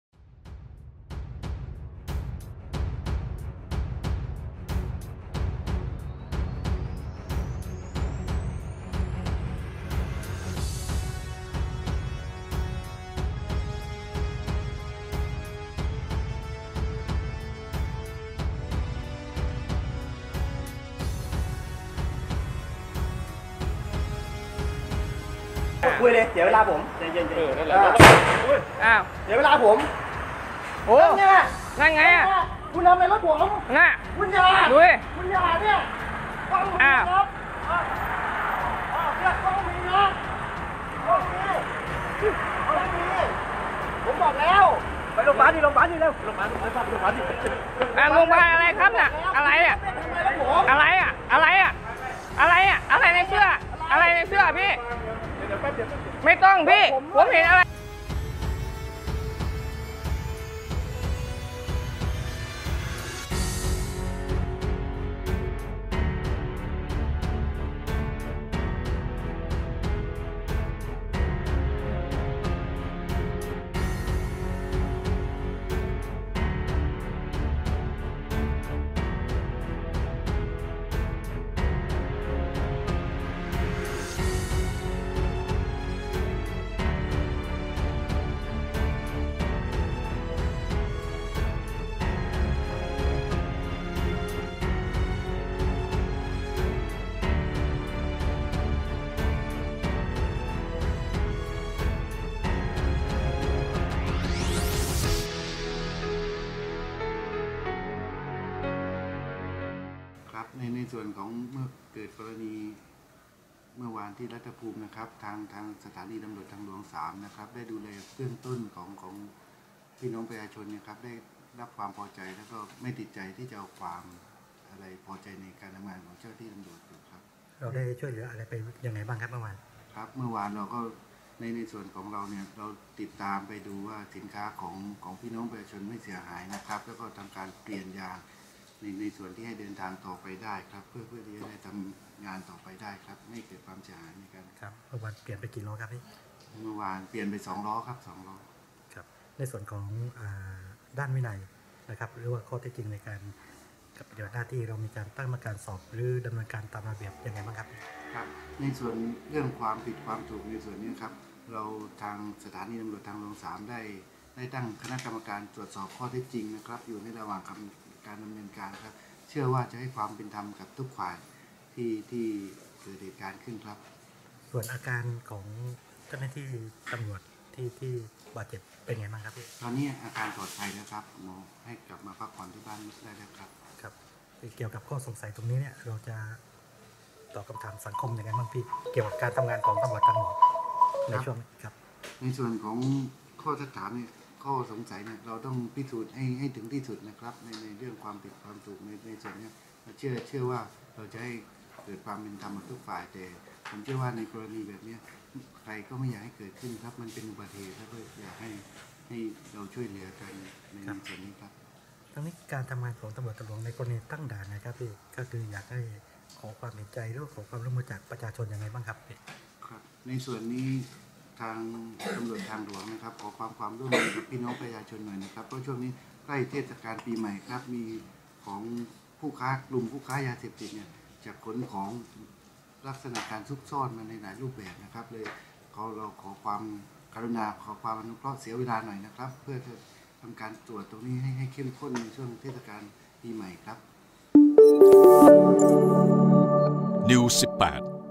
ตร.ปะทะคนขับกระบะ ยางระเบิดเฉย | NEW18
ชาวเน็ตแห่จับผิดตำรวจพกคัตเตอร์ ปะทะคารมคนขับรถกระบะ อาจกรีดยางรถจนระเบิดใส่ได้รับบาดเจ็บหรือไม่